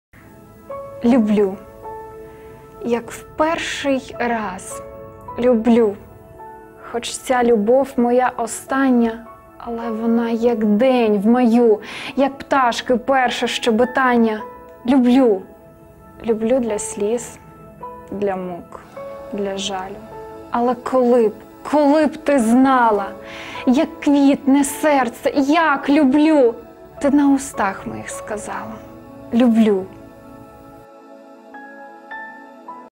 Інеса Пославська​ , вірш О. Олеся “Люблю! Як в перший раз люблю…”
Місяць української поезії на
Маєте свої поезії? Надсилайте їх на конкурс "Заримуй Рівне" на нашому сайті